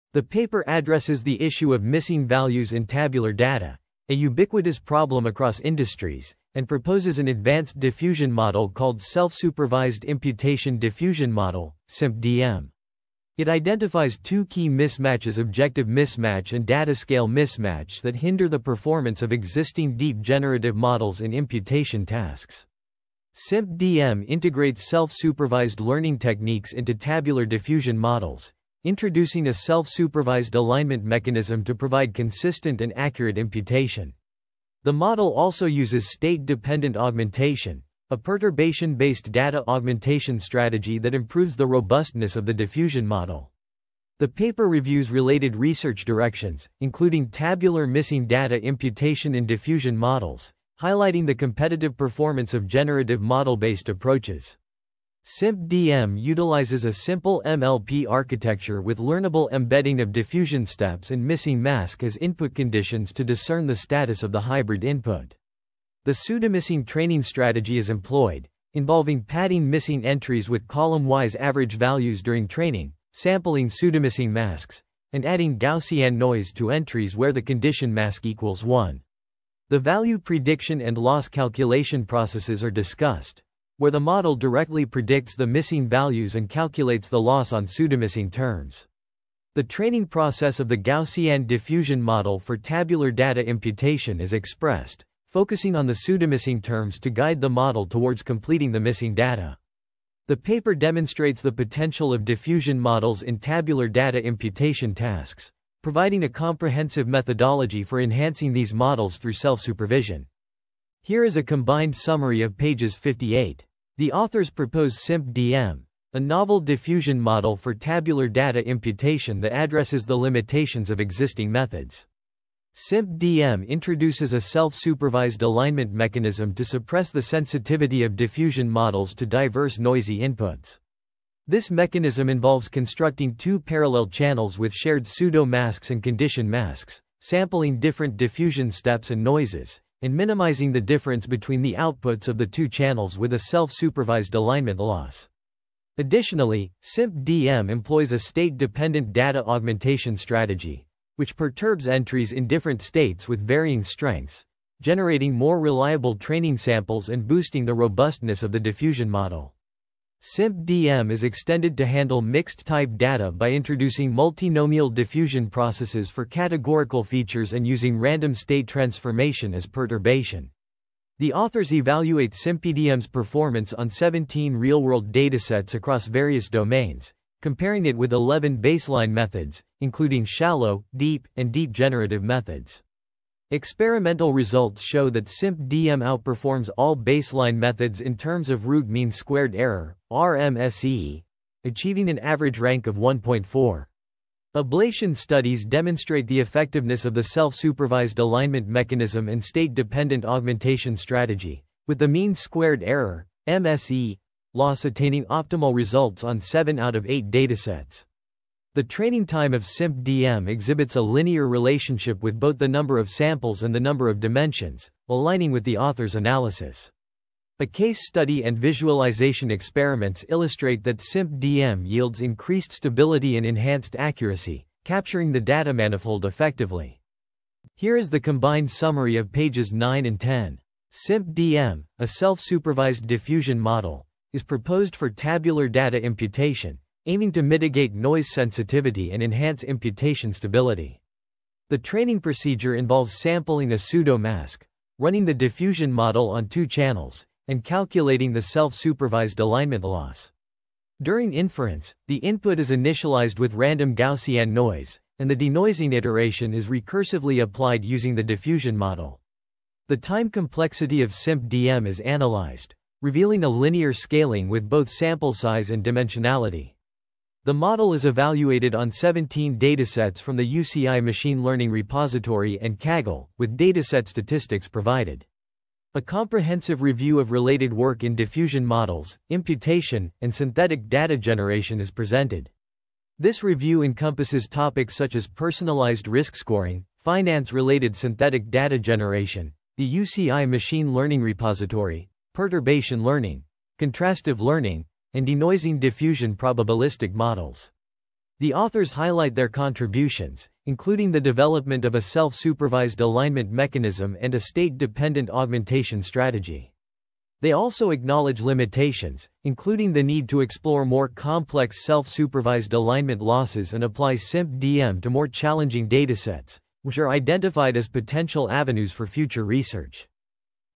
Self-Supervision Improves Diffusion Models for Tabular Data Imputation - ArXiv:2407.1801
Title: Self-Supervision Improves Diffusion Models for Tabular Data Imputation

Authors: Yixin Liu, Thalaiyasingam Ajanthan, Hisham Husain, Vu Nguyen

Abstract:
The ubiquity of missing data has sparked considerable attention and focus on tabular data imputation methods. Diffusion models, recognized as the cutting-edge technique for data generation, demonstrate significant potential in tabular data imputation tasks. However, in pursuit of diversity, vanilla diffusion models often exhibit sensitivity to initialized noises, which hinders the models from generating stable and accurate imputation results. Additionally, the sparsity inherent in tabular data poses challenges for diffusion models in accurately modeling the data manifold, impacting the robustness of these models for data imputation. To tackle these challenges, this paper introduces an advanced diffusion model named Self-supervised imputation Diffusion Model (SimpDM for brevity), specifically tailored for tabular data imputation tasks. To mitigate sensitivity to noise, we introduce a self-supervised alignment mechanism that aims to regularize the model, ensuring consistent and stable imputation predictions. Furthermore, we introduce a carefully devised state-dependent data augmentation strategy within SimpDM, enhancing the robustness of the diffusion model when dealing with limited data. Extensive experiments demonstrate that SimpDM matches or outperforms state-of-the-art imputation methods across various scenarios.